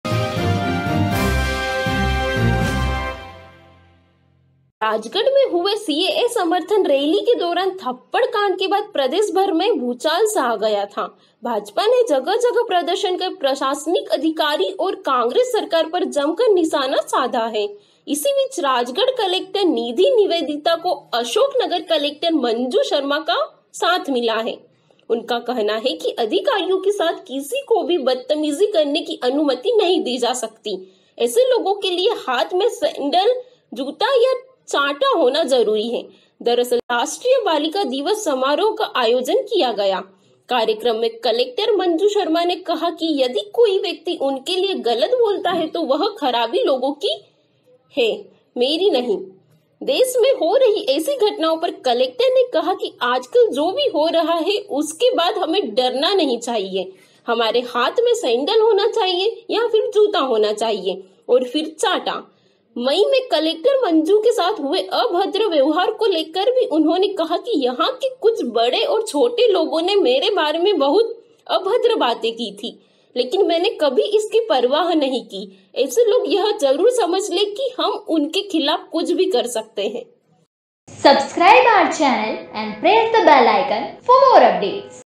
महिला कलेक्टर ने कहा बत्तमीजों के लिए तैयार रखो जूते और चप्पल
इस बयान को लेकर क्यों मच गया हंगामा